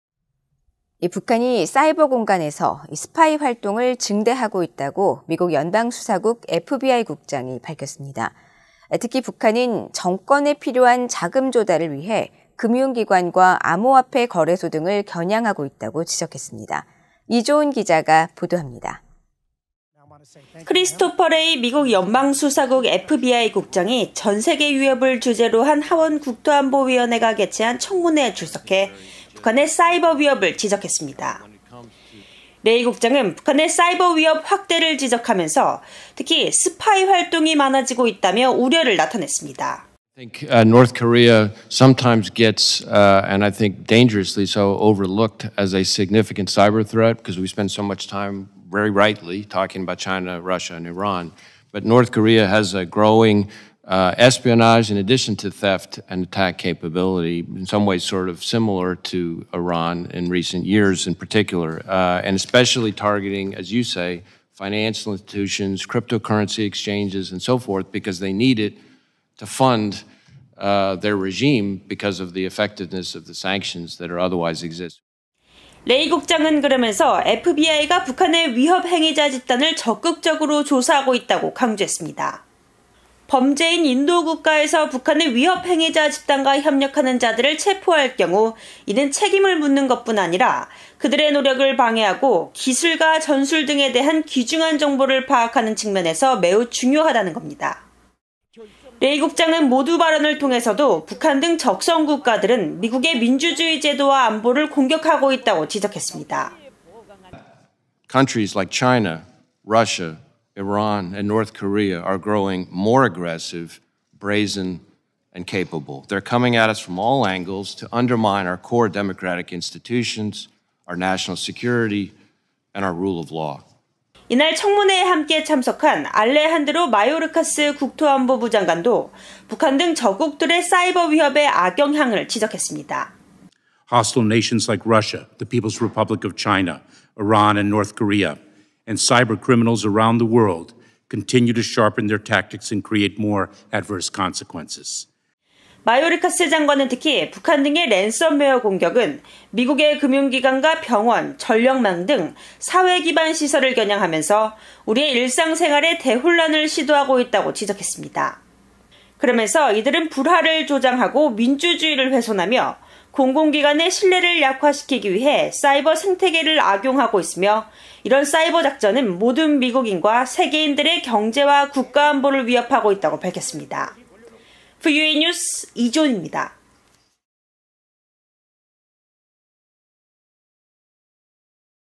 [VOA 뉴스] FBI “북한 ‘사이버 공간 스파이 활동’…‘정권 자금 조달’ 해킹”
북한이 사이버 공간에서 스파이 활동을 증대하고 있다고 미국 연방수사국 FBI 국장이 밝혔습니다. 특히 북한은 정권에 필요한 자금 조달을 위해 금융 기관과 암호화폐 거래소 등을 겨냥하고 있다고 지적했습니다. 이조은 기자가 보도합니다. (영상편집: 이상훈)